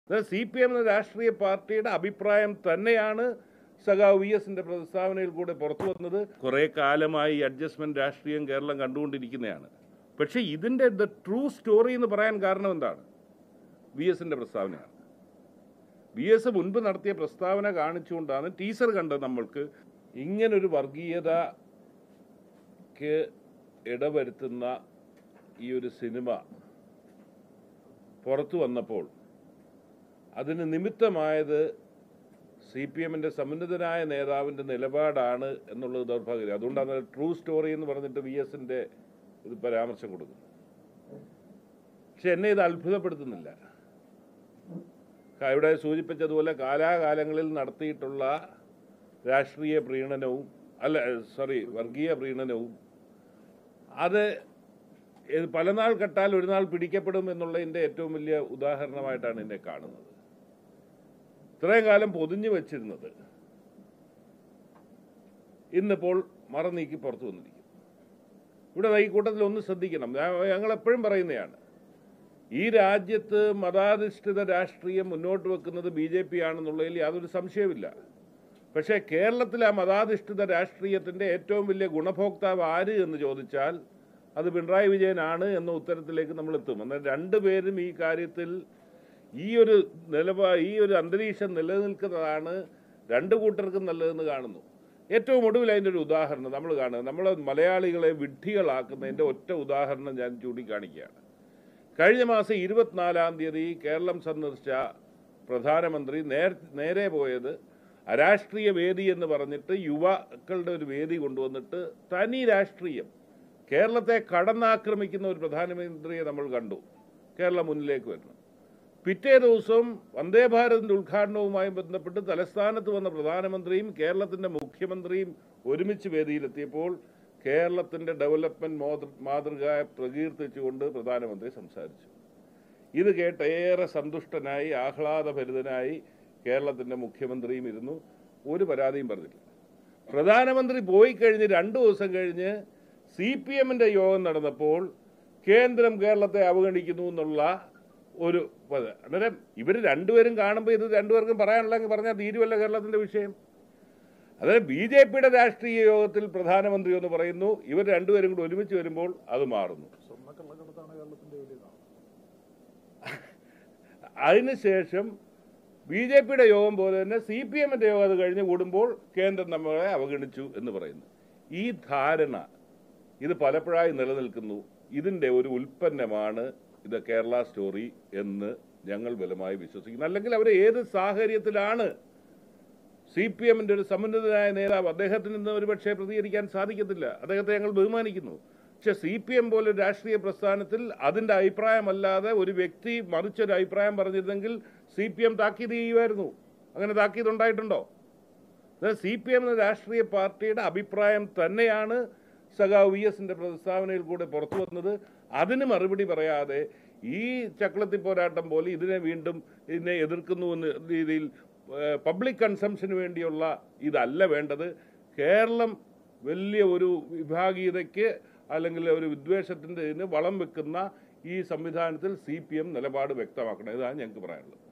കേരളത്തിലെ മതാധിഷ്ഠിത രാഷ്ട്രീയത്തിന്റെ ഏറ്റവും വലിയ ​ഗുണഭോക്താവ് പിണറായി വിജയൻ- ഷിബു ബേബി ജോൺ
സിപിഎമ്മിന്റെ നിലപാടാണ് അന്ന് വി.എസ്.അച്യുതാനന്ദന്റെ മുസ്ലിം വിരുദ്ധ പ്രസ്താവനയിലൂടെ പുറത്തുവന്നതെന്ന് ആർഎസ്പി സംസ്ഥാന സെക്രട്ടറി ഷിബു ബേബി ജോൺ

Happy viewing!